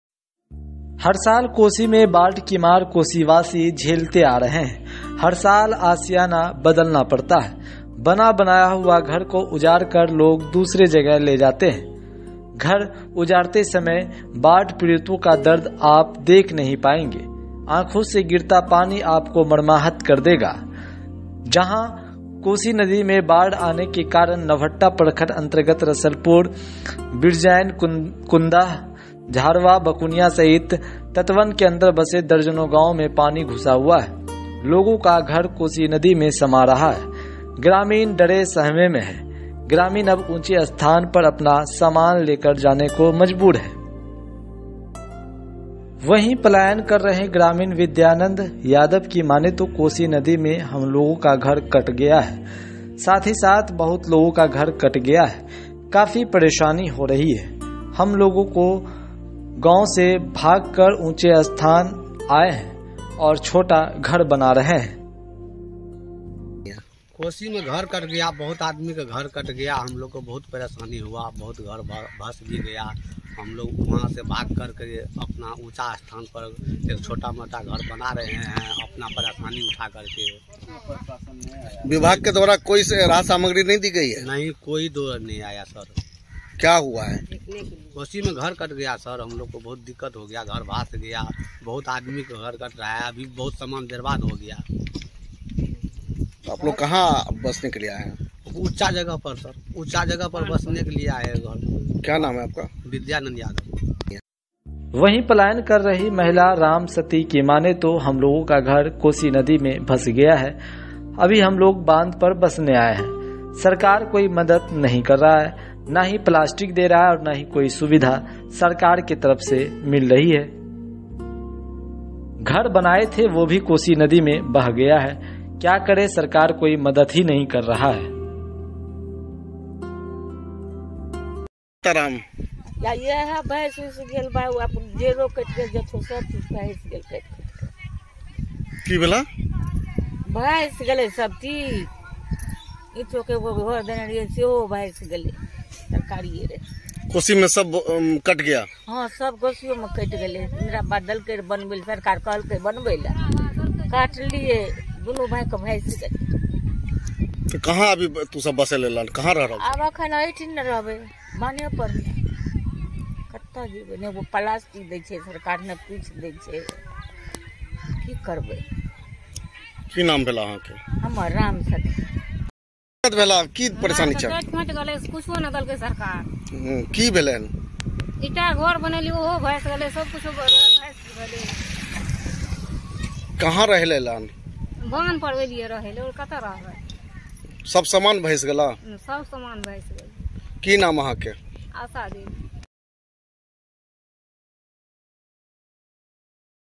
Bihar Flood News: Sahasra में Kosi नदी का तांडव, बाढ़ से लोग अपने घर तोड़ने को हुए मजबूर!
Due to flood in Kosi River, dozens of villages situated inside the embankment including Rasalpur, Birgen, Kundah, Jharwa, Bakuniyan under Navahatta block are under water. People's houses are getting drowned in Kosi River. Villagers are scared. Villagers are now on high ground. He is forced to carry his luggage to the place. According to Vidyanand Yadav, a villager who is migrating, our house has been destroyed in the Kosi River and at the same time the houses of many people have been destroyed. We are facing a lot of trouble. We have fled from the village to a higher place and the small house Making.

कोशी नदी में बाढ़ आने के कारण नवहट्टा प्रखंड अंतर्गत रसलपुर, बिरजेन, कुंदह, झारवा, बकुनियाँ, सहित सहित तटबंध के अंदर बसे दर्जनों गांव  में पानी घुसा हुआ है लोगों का घर कोशी नदी में समा रहा है।ग्रामीण डरे सहमे हैं ग्रामीण अब ऊंचे स्थान पर अपना सामान लेकर जाने को मजबूर है। वहीं पलायन कर रहे ग्रामीण विद्यानंद यादव की माने तो कोशी नदी में हमलोगों का घर कट गया है साथ ही साथ बहुत लोगों का घर कट गया है।काफी परेशानी हो रही है हमलोगों को ।हमलोग गांव से भागकर ऊंचा स्थान पर आए हैं और छोटा घर बना रहे हैं।